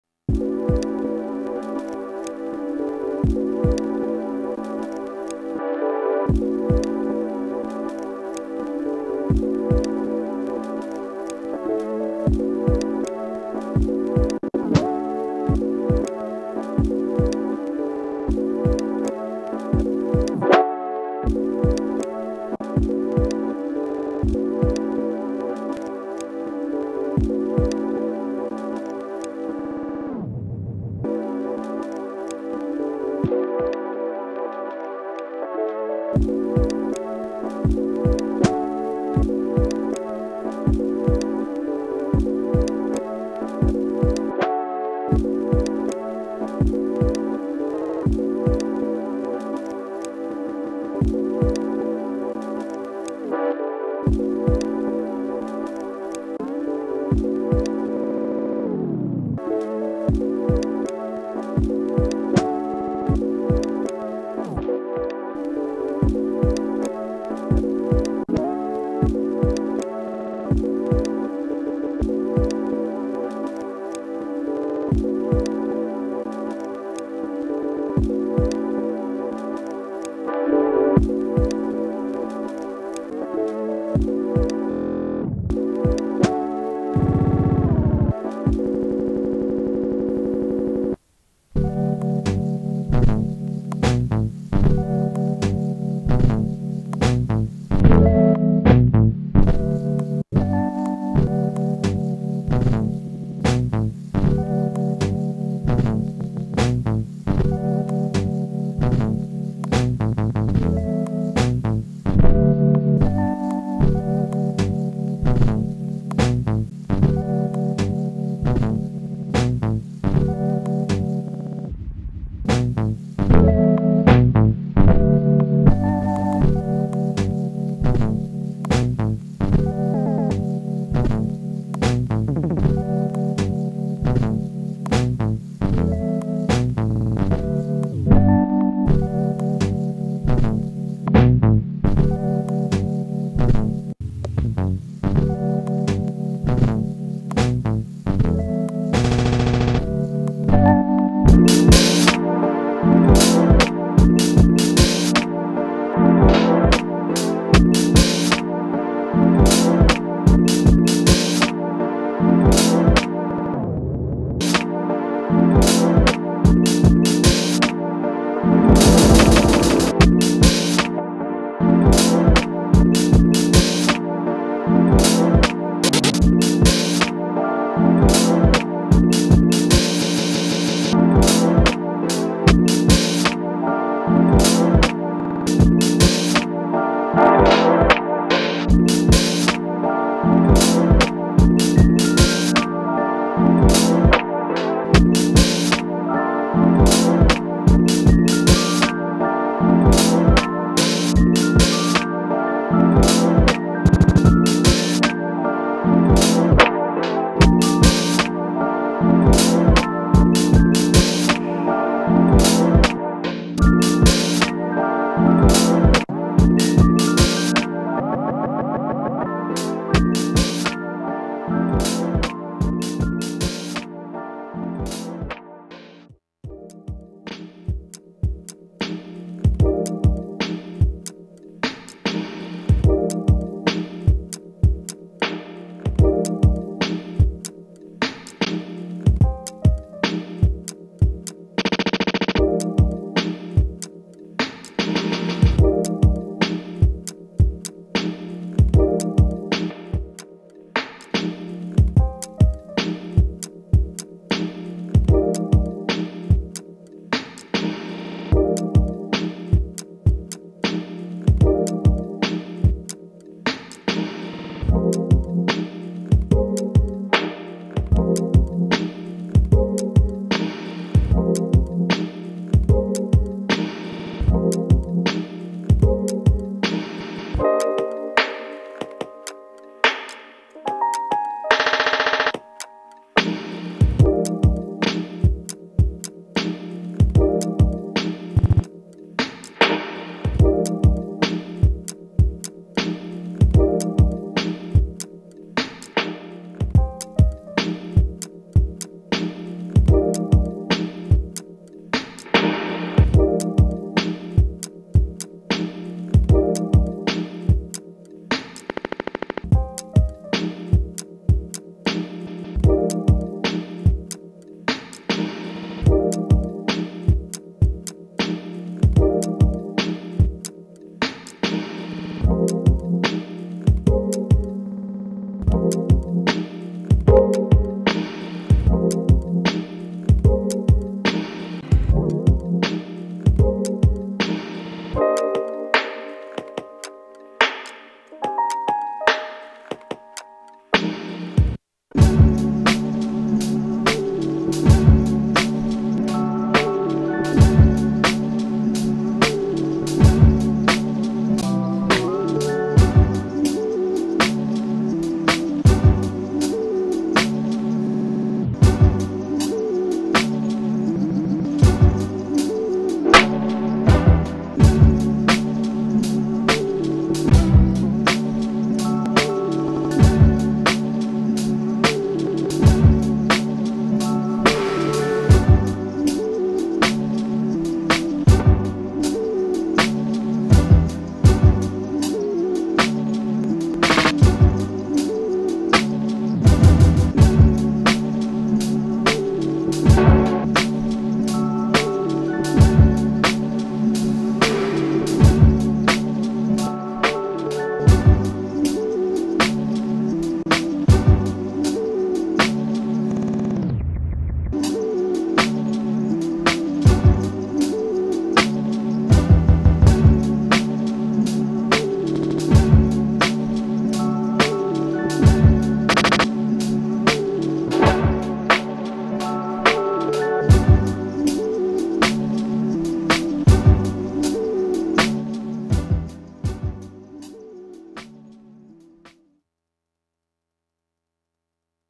Fuzzy Bunny Slippers meets the Outdoors
Live set of some songs from my next beattape